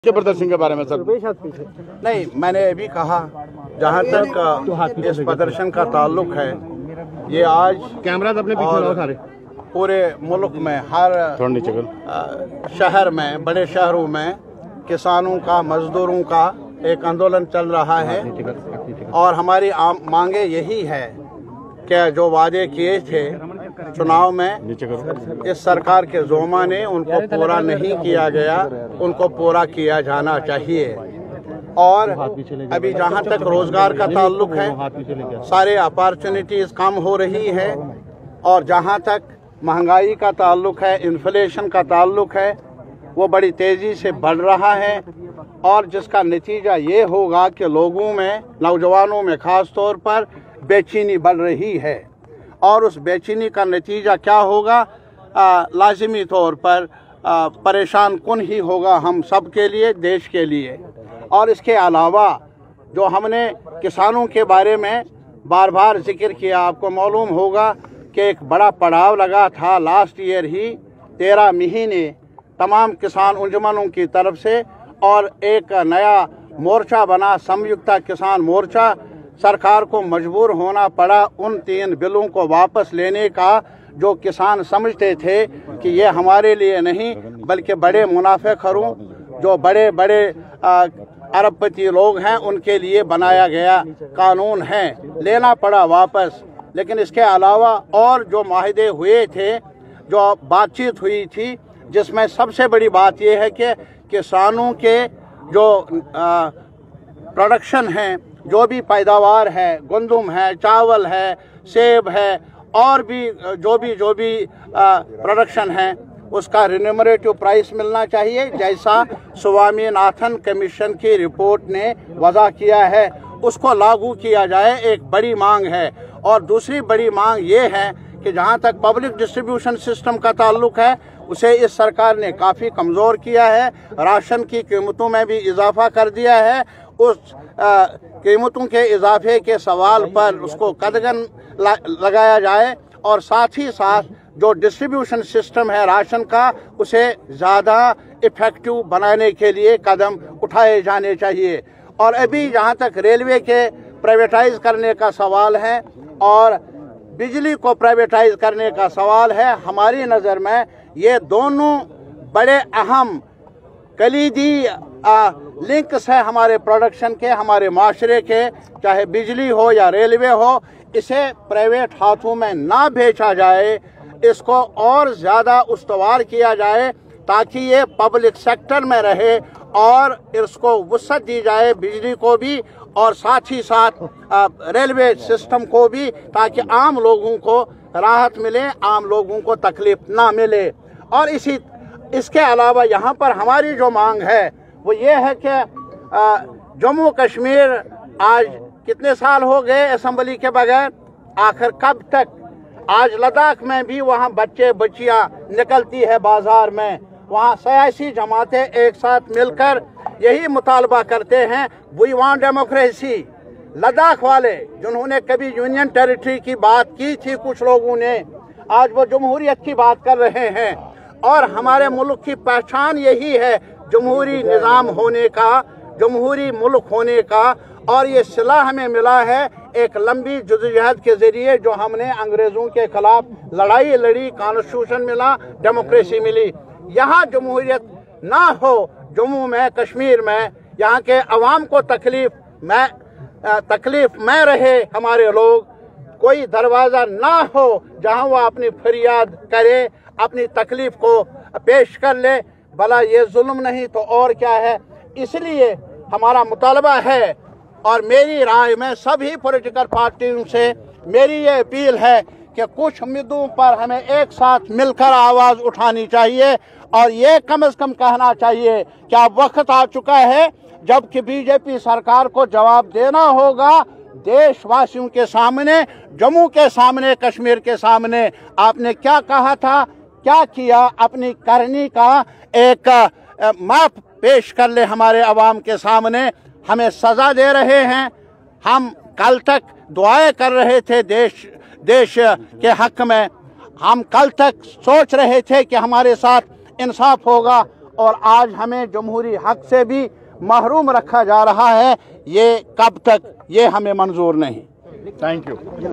JLKMEDIA Mohammed Yousuf Tarigami reached at Hari Singh Park to support CITU
jlkmedia Mohammed Yousuf Tarigami reached at Hari Singh Park to support CITU and JK Kisaan Tehreek Activists.